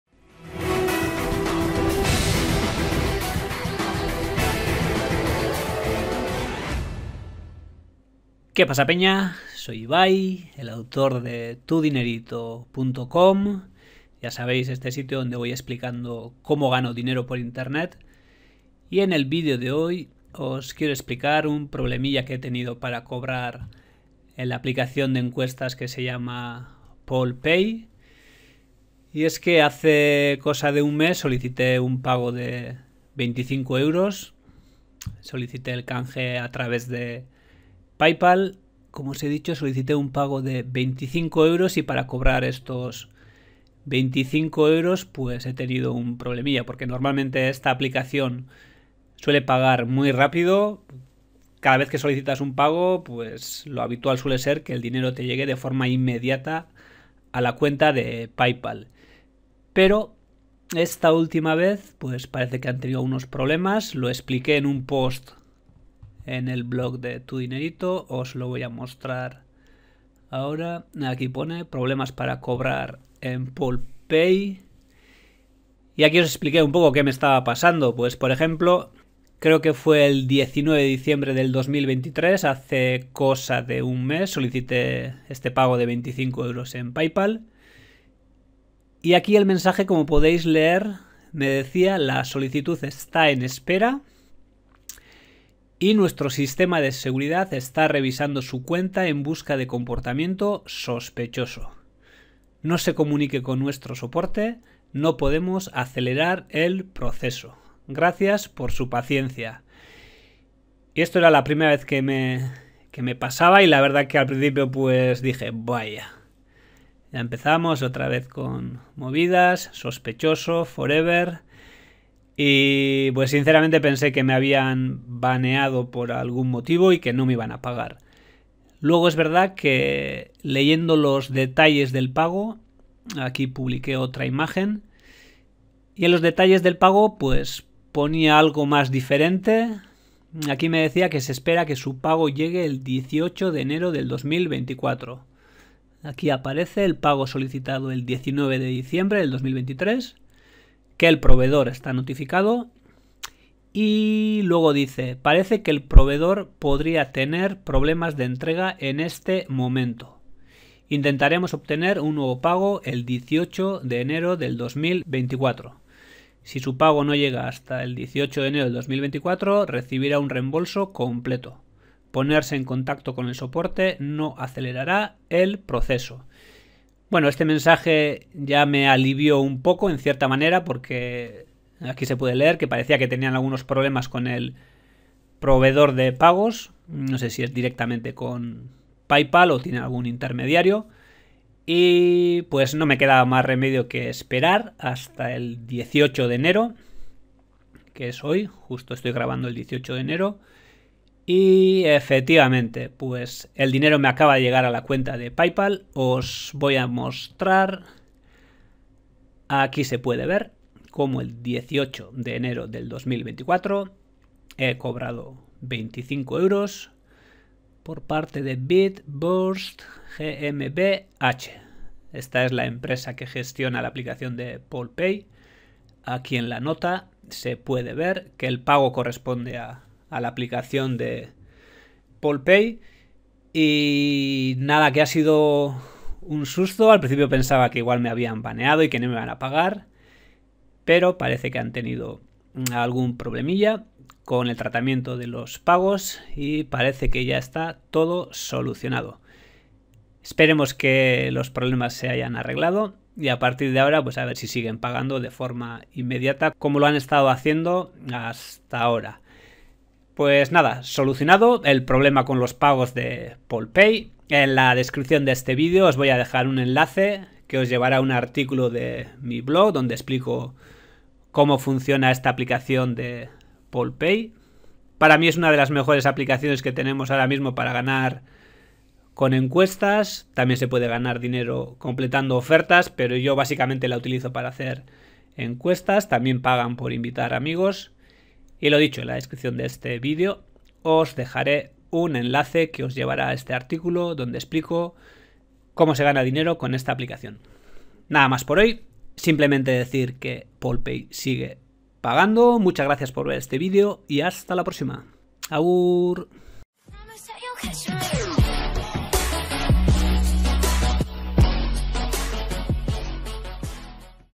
Problema RESUELTO para cobrar 25€ en PollPay
PollPay es una app para ganar dinero haciendo encuestas. PollPay funciona para España y México. Normalmente PollPay suele pagar de forma inmediata. Sí, canjeas un premio en Paypal y el dinero te llega de forma instantánea a la cuenta de PayPal. Pero esta vez he tenido un problemilla...

Pero por suerte se ha solucionado tal como he explicado en el vídeo. :)